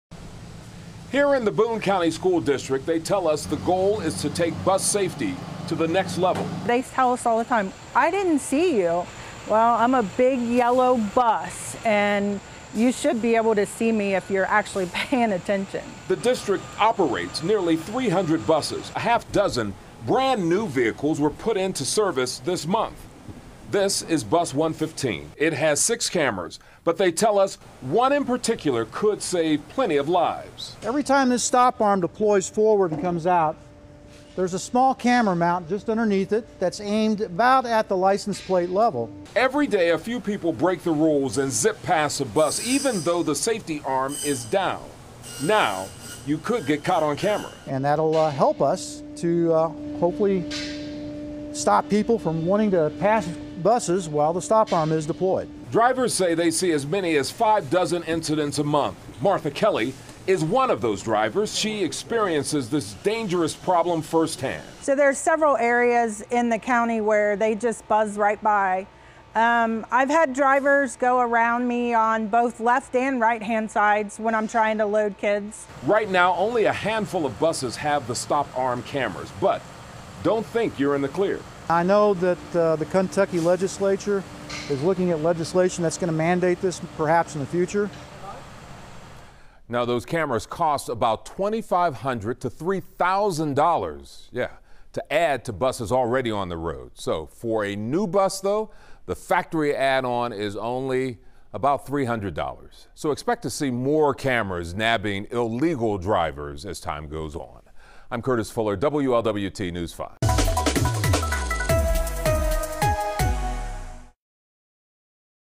NKY school district gets fleet of buses with stop-arm cameras
In an attempt to crack down on drivers illegally passing school buses, a Northern Kentucky school district has added cameras.